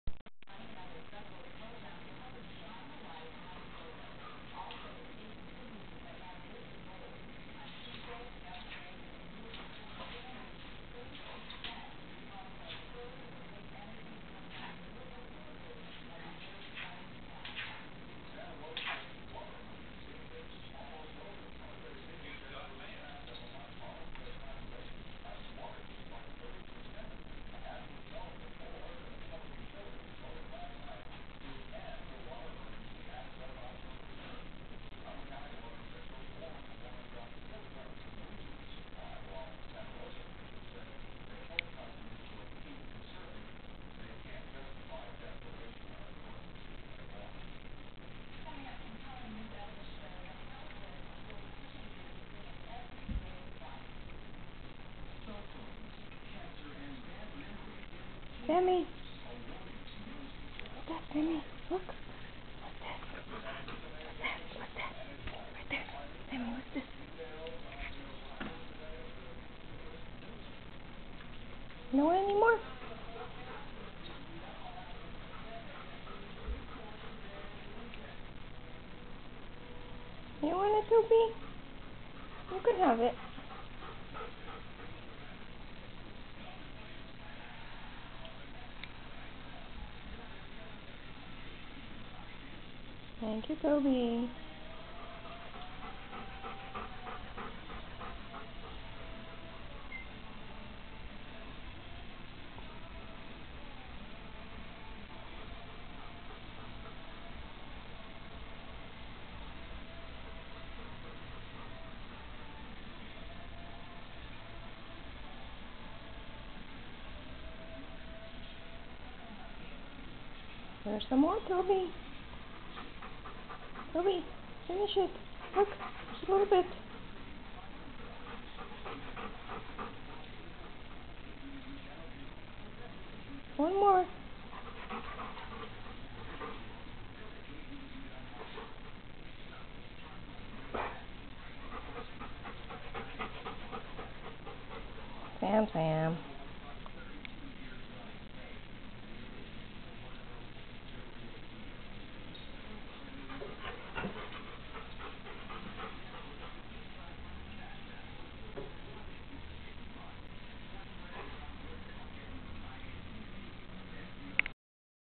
Watermelon
This video is dedicated to our beloved four-legged friend Sammy the Airedale...better known to us as 'Sammy Bear'.  Despite his sickness of diabetes, not once did he show pain and lack of energy.  Everyday in the best way he can Sammy gave us unconditional love until the end.  Rest in Peace Sammy Bear...5/2/11.  Forever our best friend.